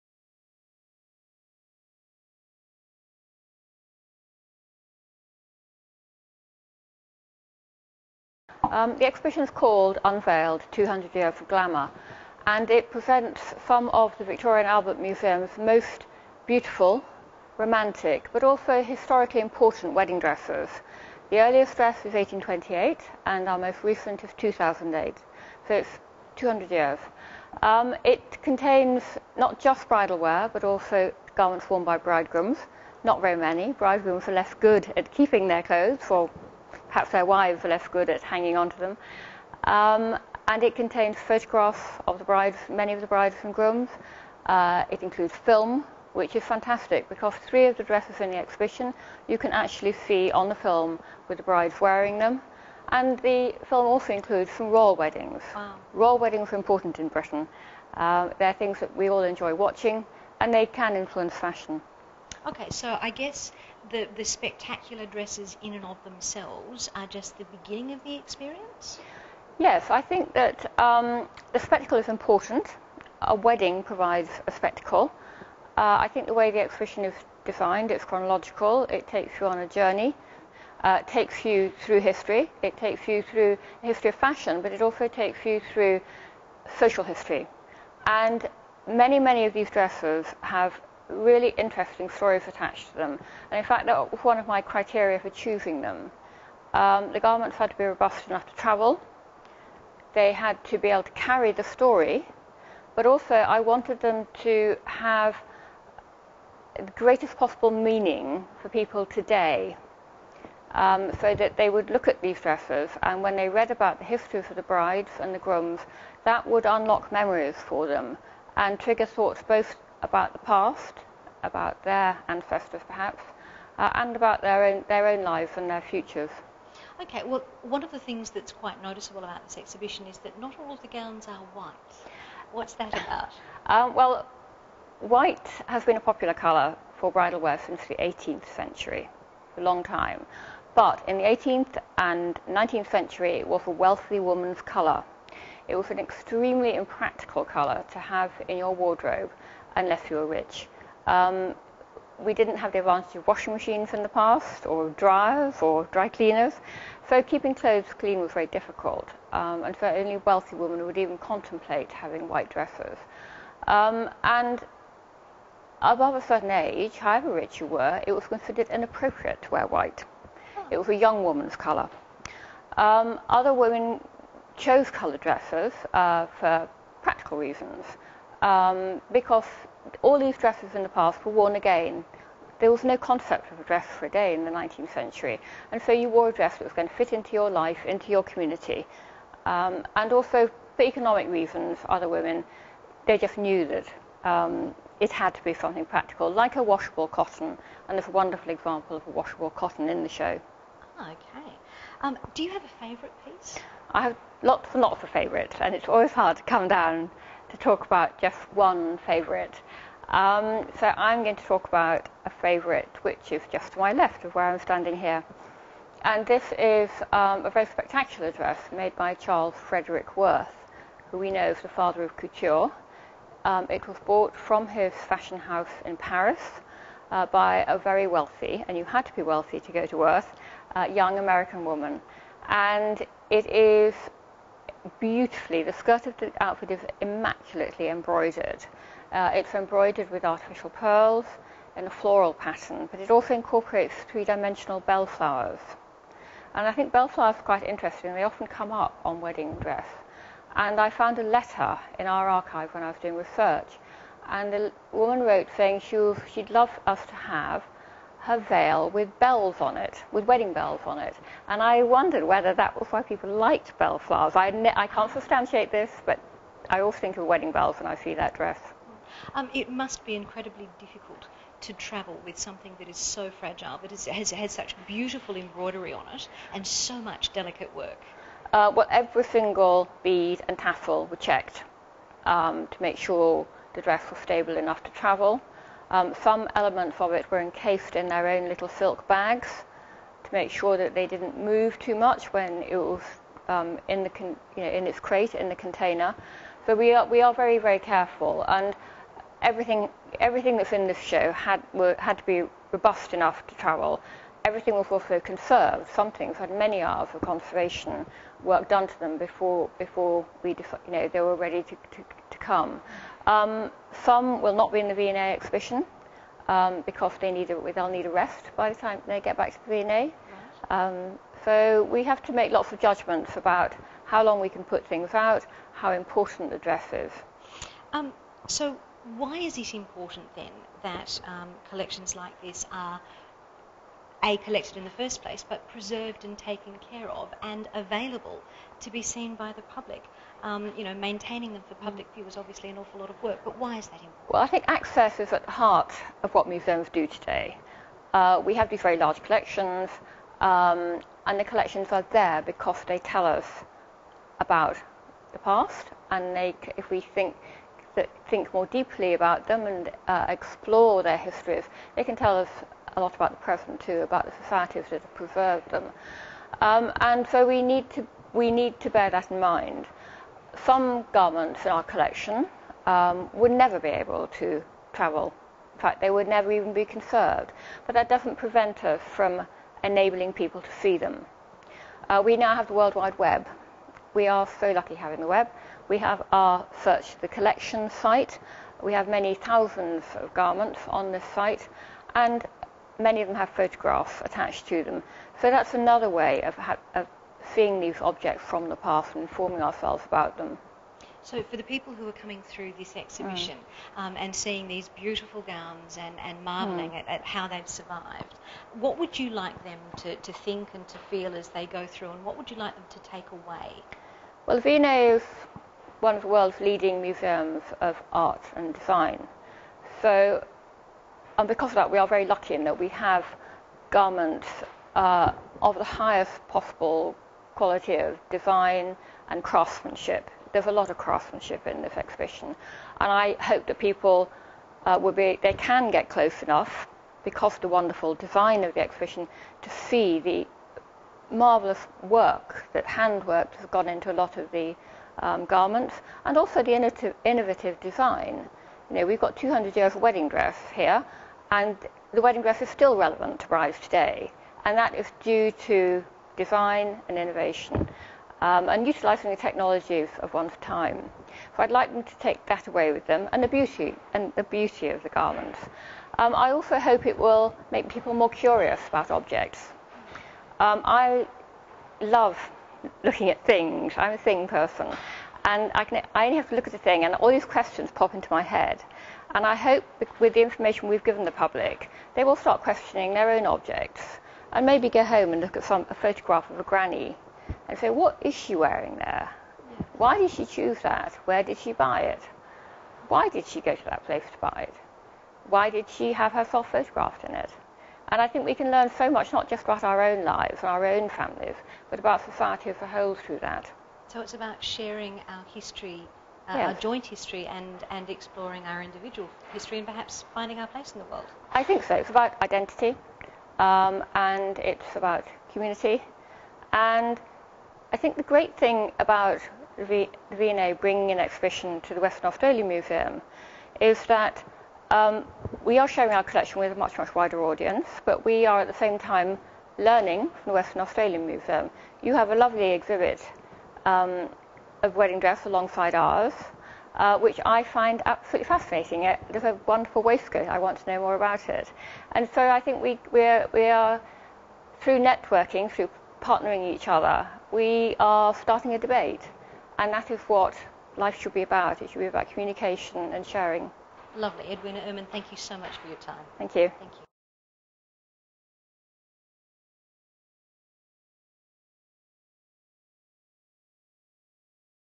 Interview with Unveiled curator Edwina Ehrman
This interview is with Victoria and Albert Museum curator Edwina Ehrman, who curated the exhibition Unveiled: 200 years of wedding glamour, on show at the Western Australian Museum – Perth from 8 December 2012 to 2 April 2013.